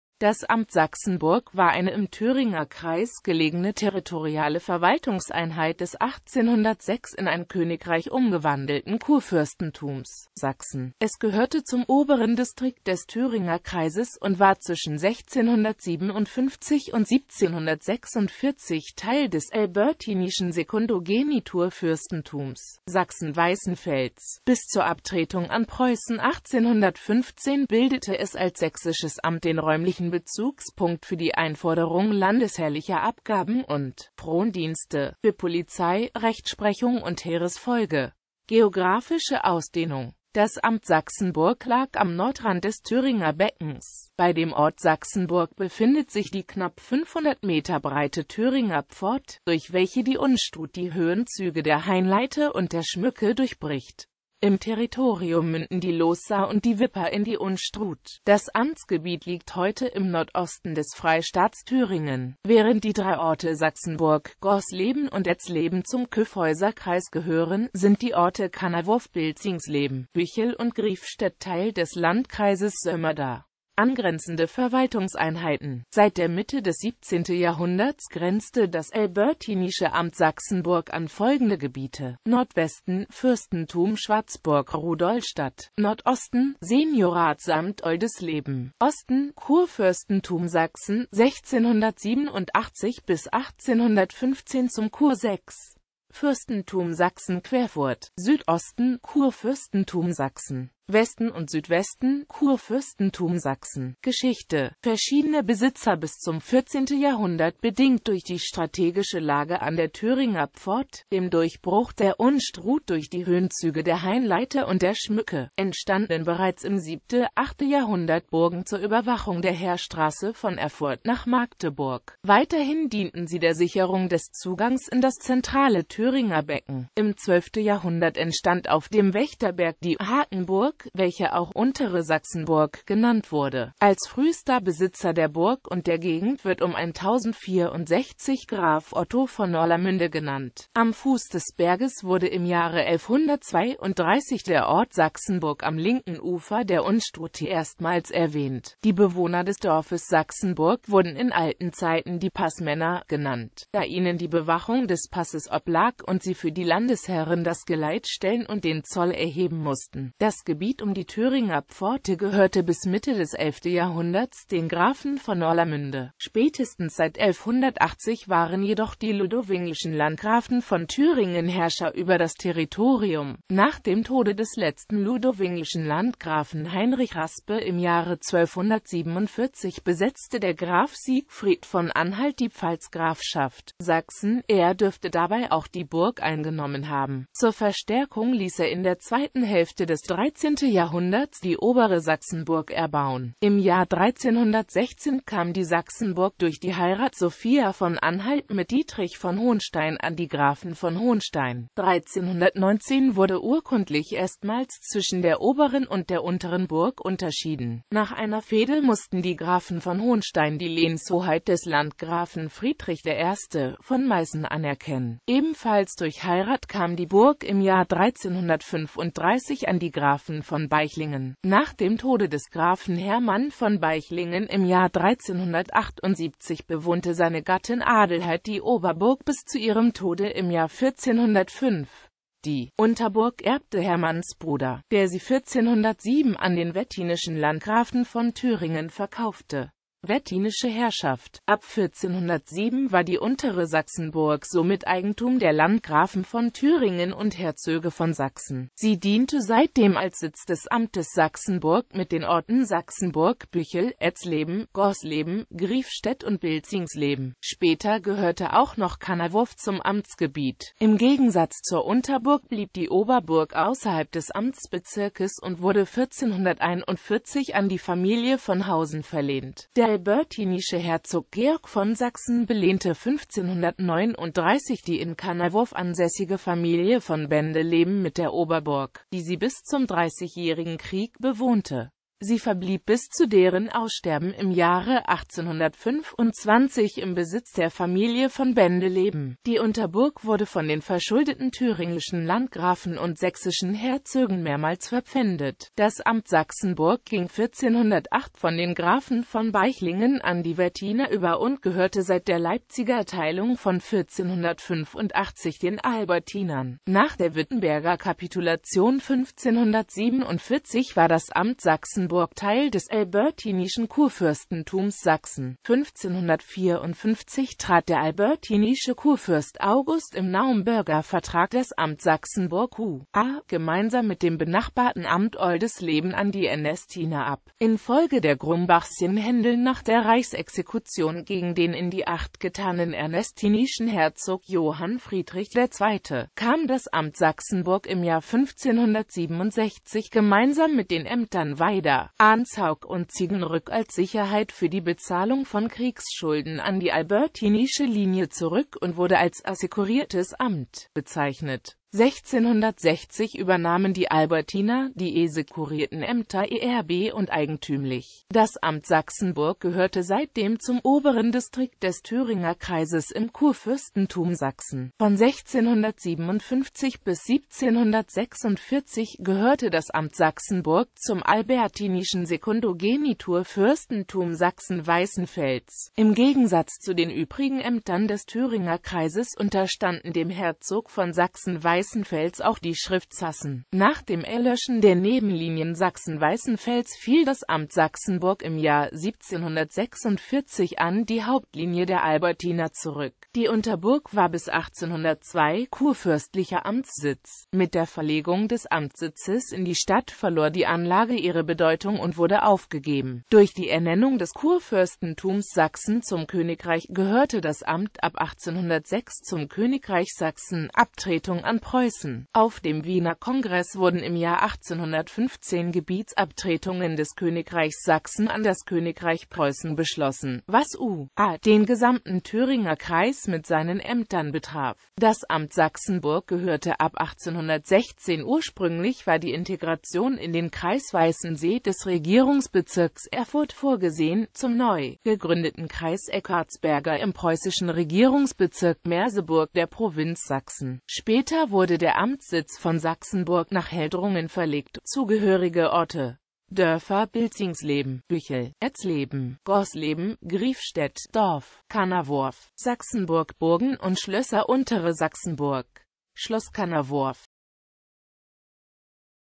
Amt Sachsenburg
Vielen Dank für Ihre Unterstützung:

Das Amt Sachsenburg war eine im Thüringer Kreis gelegene territoriale Verwaltungseinheit des 1806 in ein Königreich umgewandelten Kurfürstentums Sachsen.Es gehörte zum „Oberen Distrikt“ des Thüringer Kreises und war zwischen 1657 und 1746 Teil des albertinischen Sekundogenitur-Fürstentums Sachsen-Weißenfels.Bis zur Abtretung an Preußen 1815 bildete es als sächsisches Amt den räumlichen Bezugspunkt für die Einforderung landesherrlicher Abgaben und Frondienste, für Polizei, Rechtsprechung und Heeresfolge.

Bild-Copyright-Informationen
Urheber Info: Michael Sander